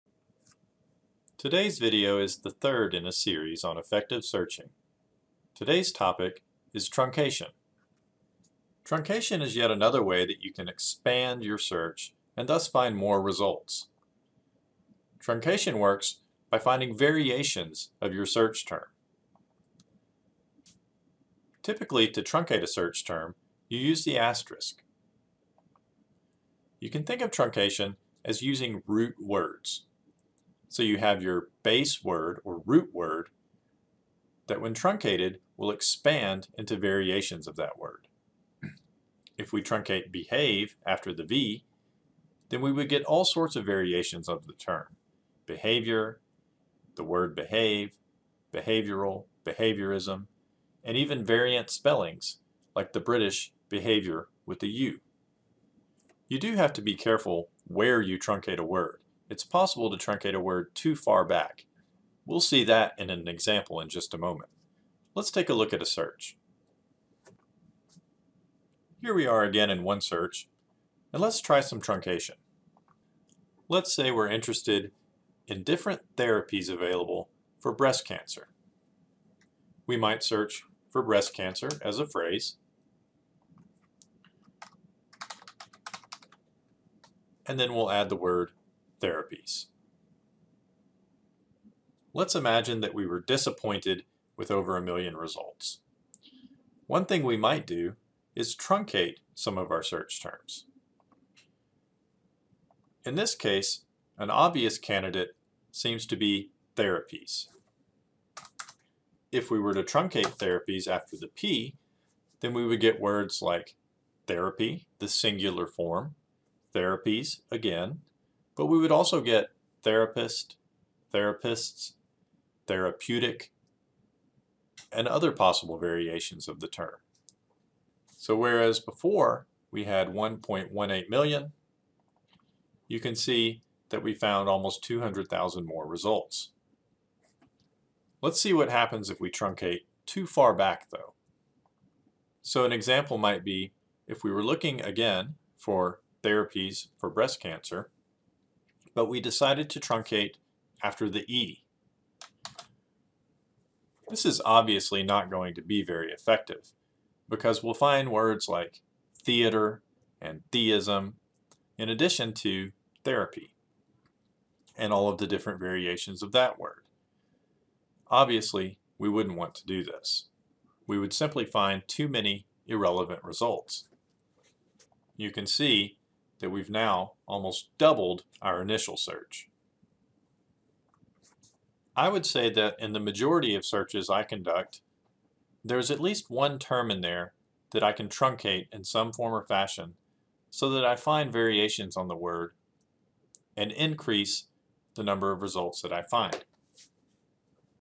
Effective Searching: Truncation
Learn how to use truncation to improve the quality of your searches.